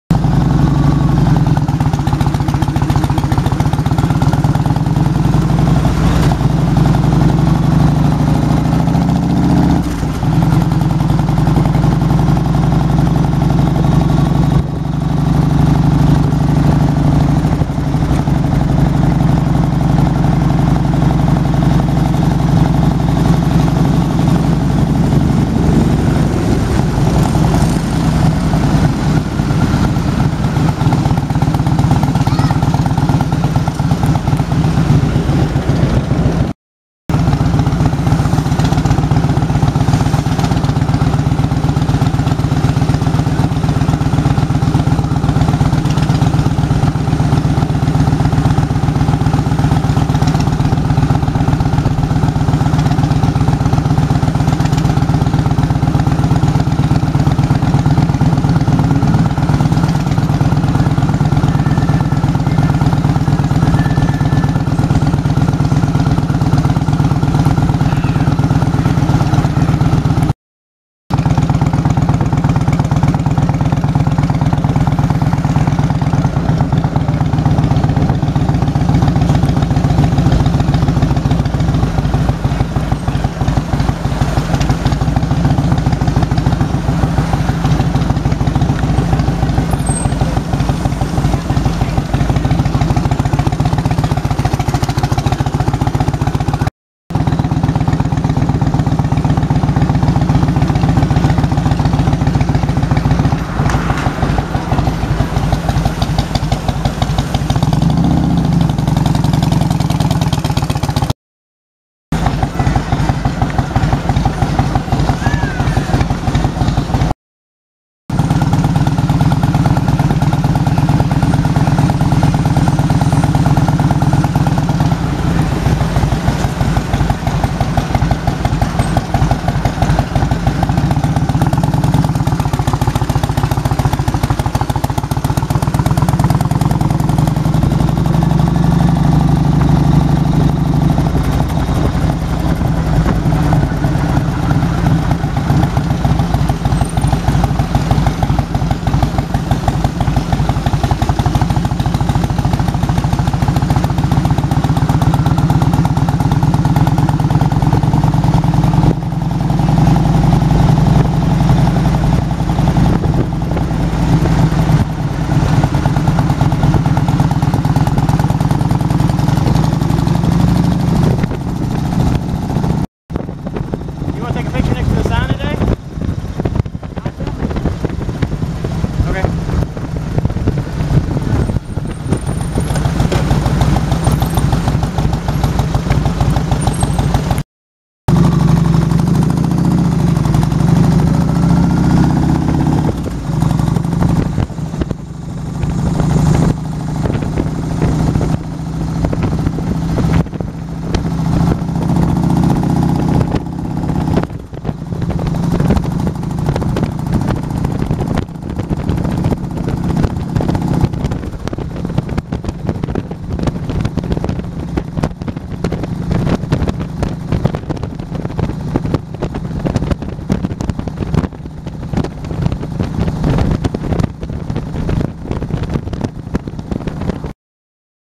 Downtown Gatlinburg Tenessee
My beautiful wife is helping me film, she thought you'd all like to see it from the back of a bike.